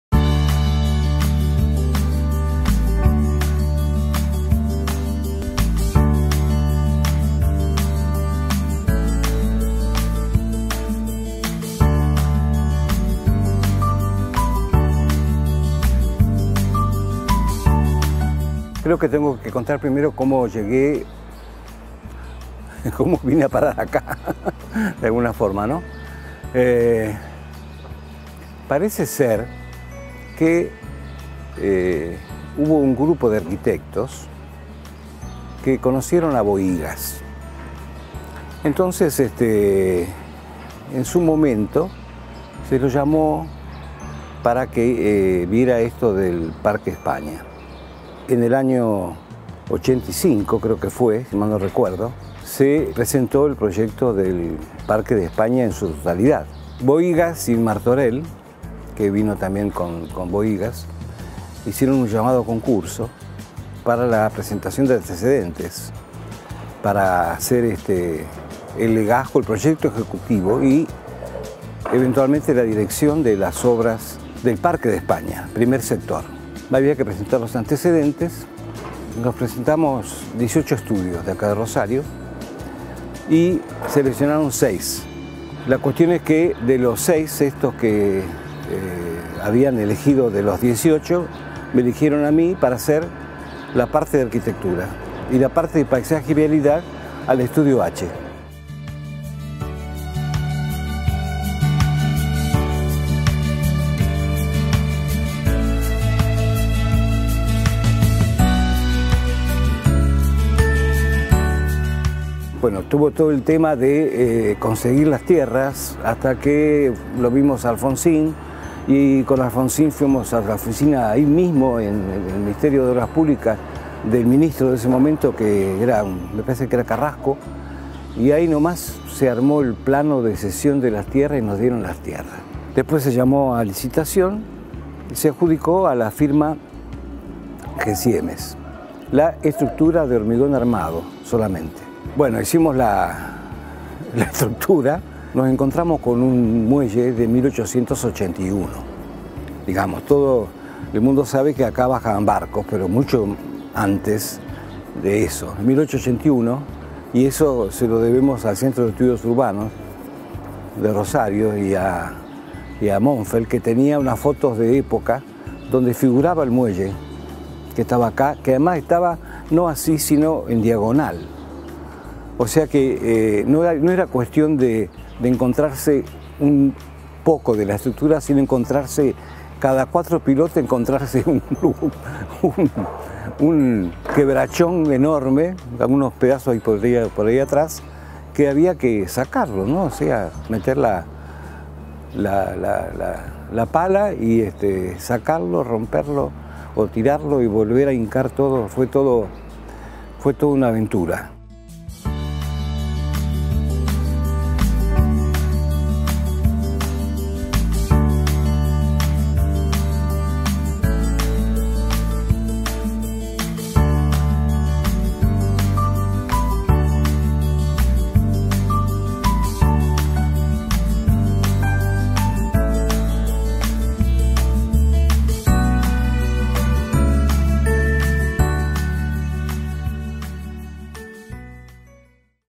Circuito do Porto_Parada 7: Parque da Espanha
O arquiteto Horario Quiroga conta como foi a experiência de dirigir as obras do Centro Cultural Parque de España.